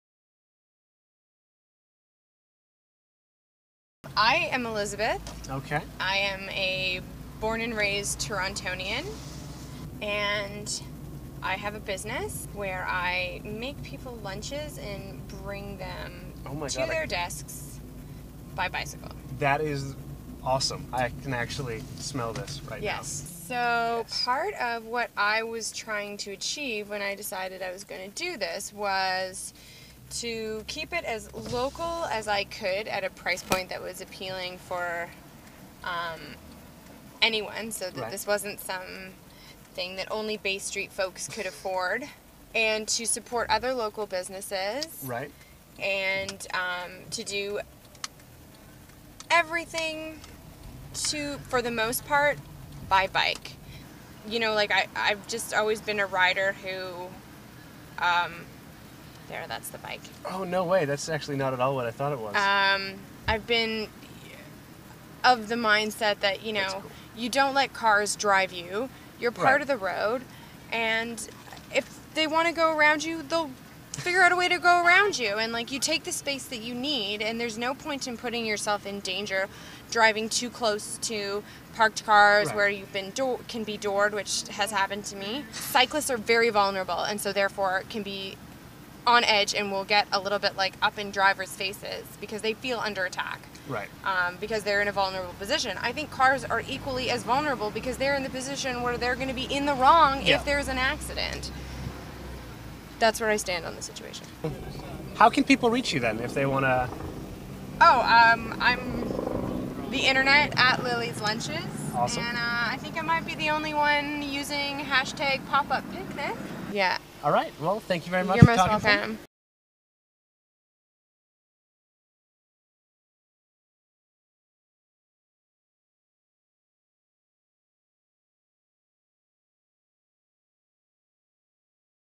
Driving through Kensington with Lilly
I give Elizabeth, of Lilly's Lunches, a ride to pick up baking pans in Kensington market. She runs a business for which she delivers lunches by bike to offices around the city. Needless to say she has interesting things to say about biking in downtown Toronto.